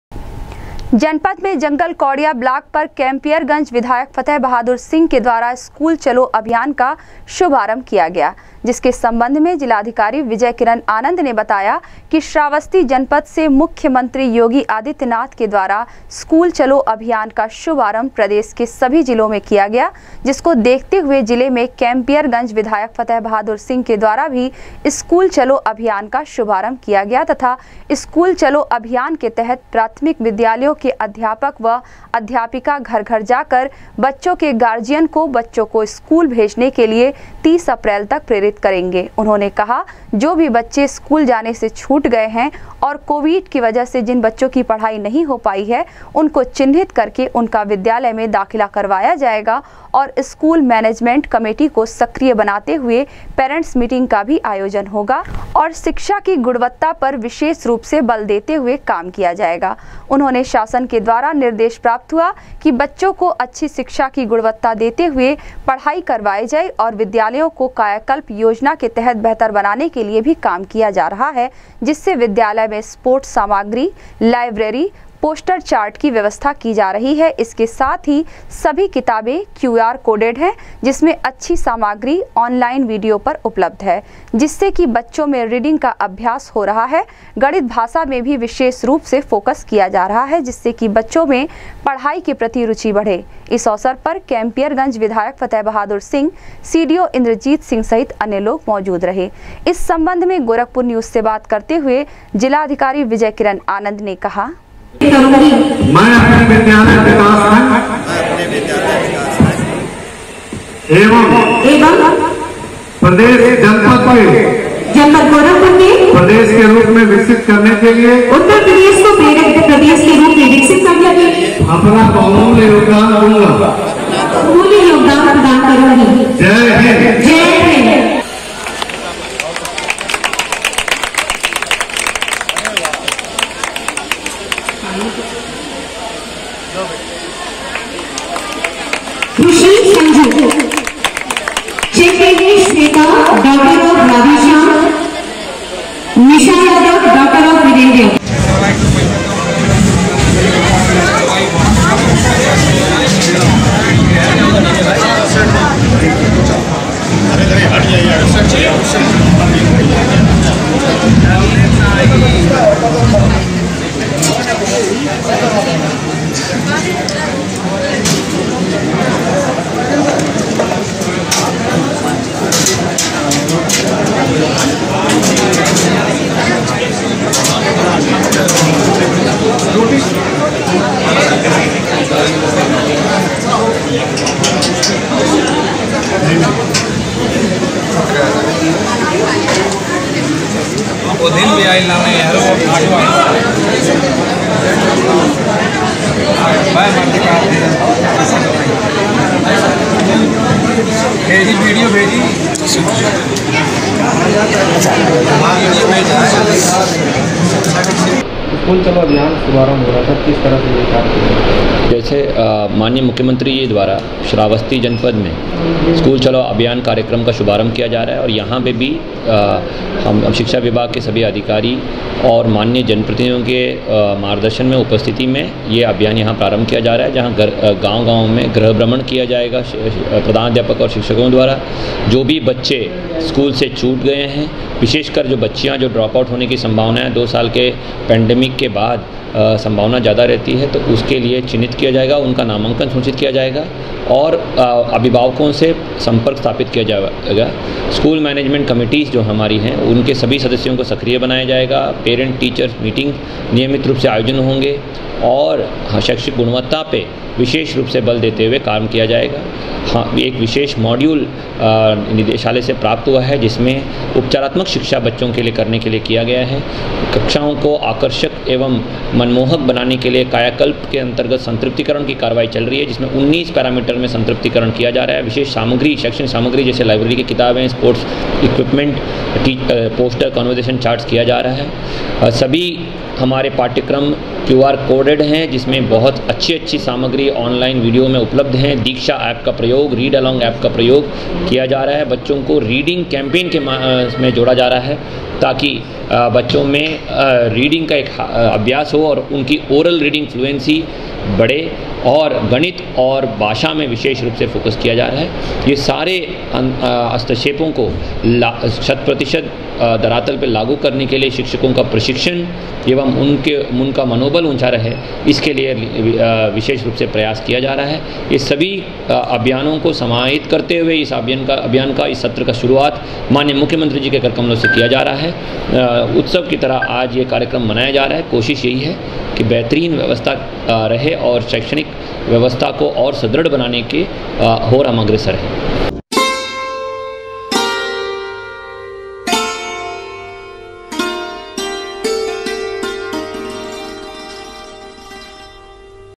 गोरखपुर में स्कूल चलो अभियान का जंगल कौड़िया ब्लाक पर कैंम्पियरगंज के विधायक फतेह बहादुर सिंह के
गोरखपुर में स्कूल चलो अभियान का जंगल कौड़िया ब्लाक पर कैंम्पियरगंज के विधायक फतेह बहादुर सिंह के द्वारा किया गया शुभारंभ,जिलाधिकारी ने कहा, स्कूल चलो अभियान के तहत विद्यालय के अध्यापक व अध्यापिका घर-घर जाकर बच्चो के गार्जियन को बच्चो को स्कूल भेजने के लिए करेंगी प्रेरित